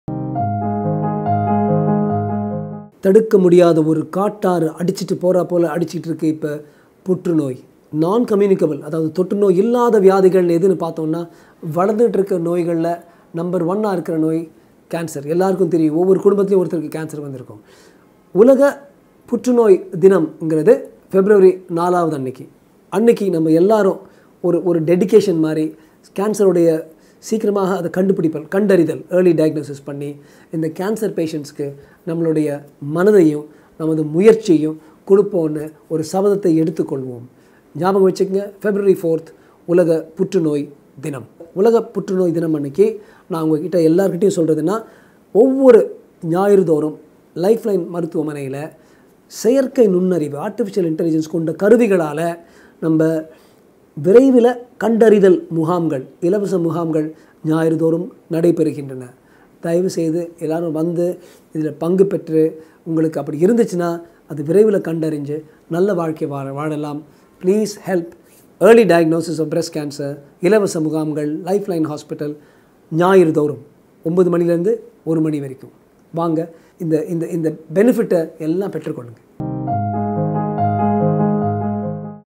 World Cancer Day : Inspiring Fight Against Cancer I Dr J S Rajkumar I LifeLine Hospitals
Join us on World Cancer Day as we raise awareness and show support for those affected by breast cancer. This powerful and inspiring speech highlights the importance of early detection, treatment options, and the ongoing fight against this disease. Let's come together to show our support and help find a cure.